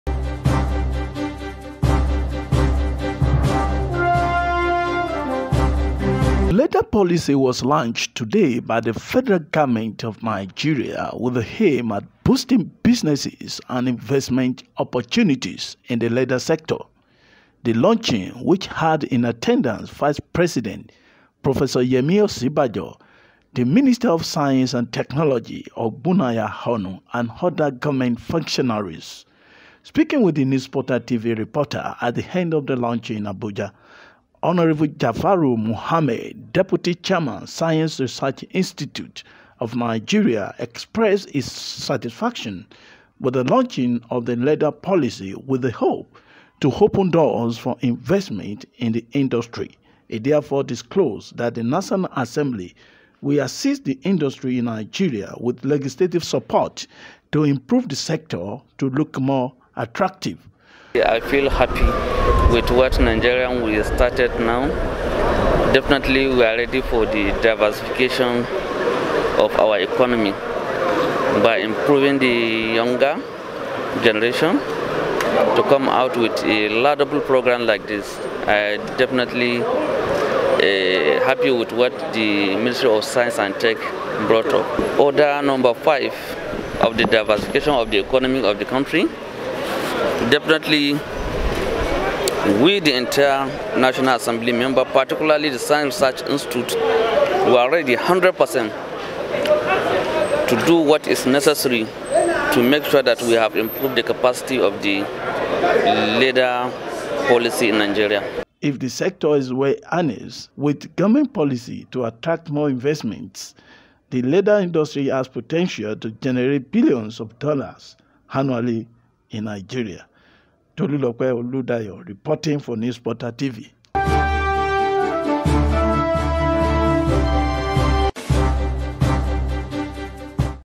Hon. Jafaru Mohammed, D/C Sci. Rech Inst. Promises Legislative Support 2 Harness Leather Ind in Nig
Hon. Jafaru Mohammed, Deputy Chairman, Science Research Institute of Nigeria has promised Legislative Support for the turn around of Leather sector through Bills and collaboration with stakeholders to attract more investments in the industry